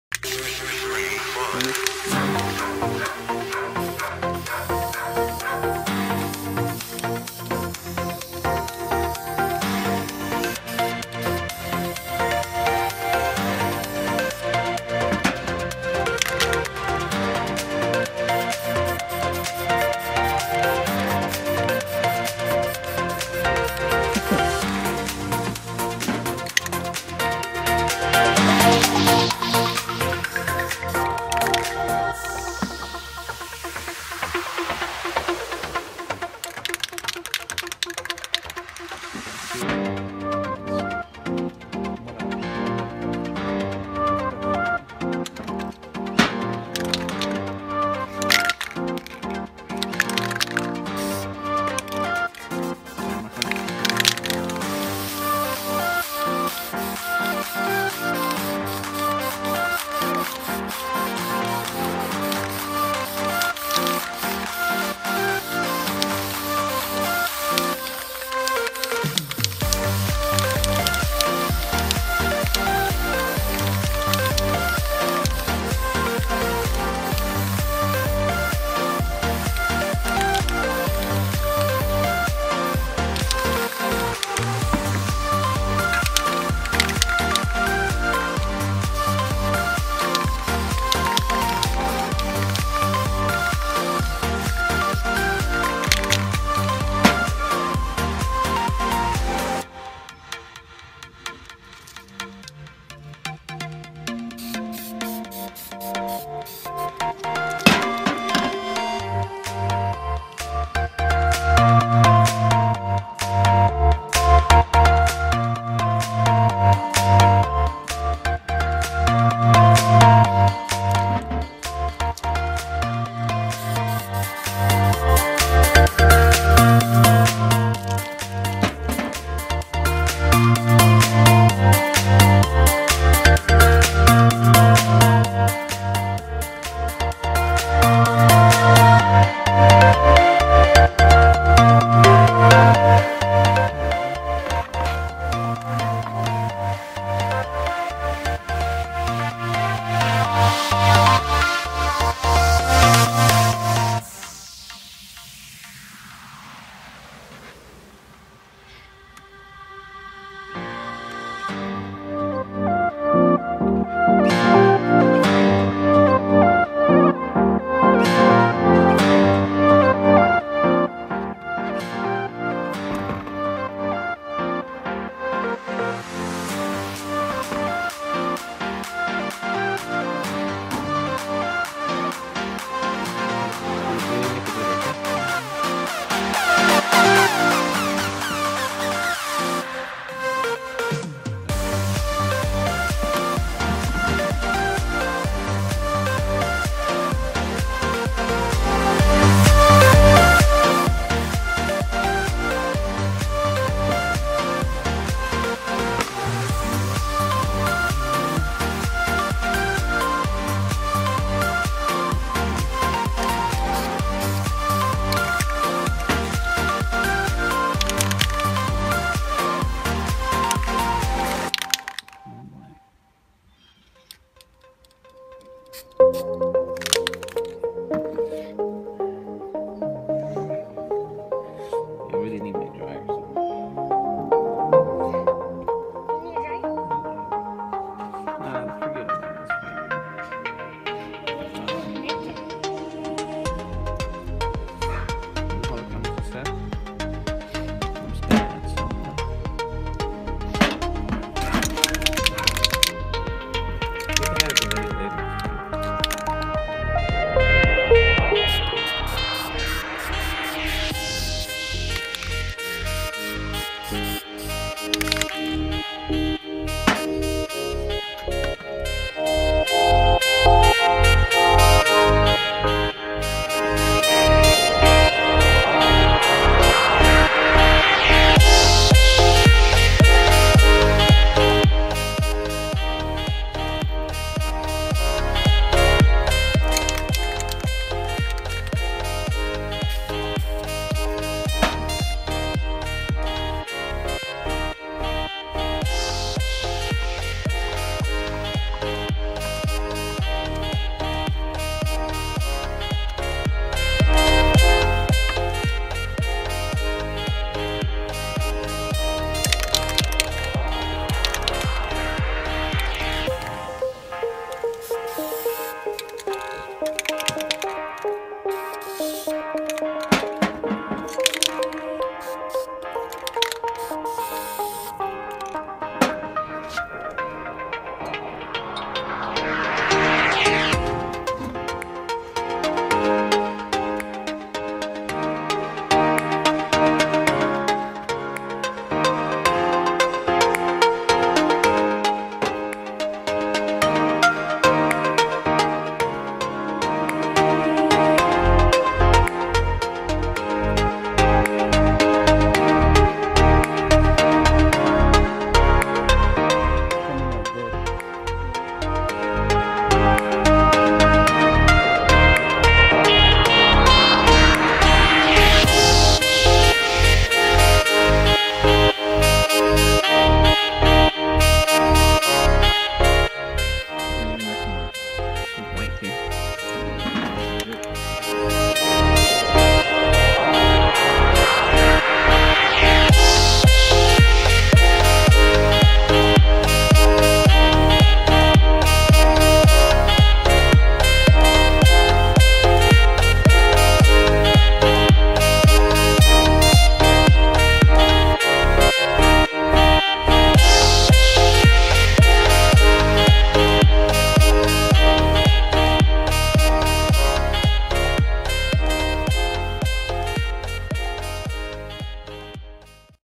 Amazing Spray Paint Art | Exotic Flower
Purple flower requested by my little sister in law